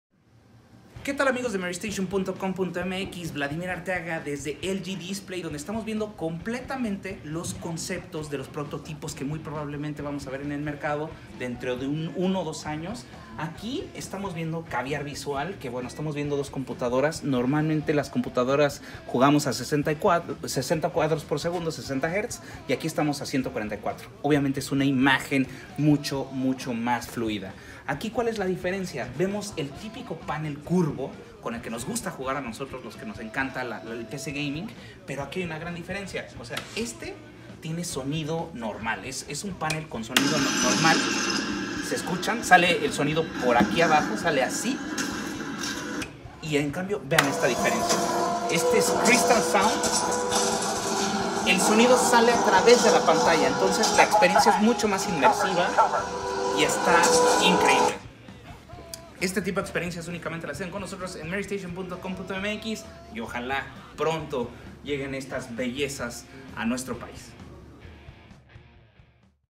CES 2018: Monitores LG con Crystal sound
Este será el futuro de los monitores para gaming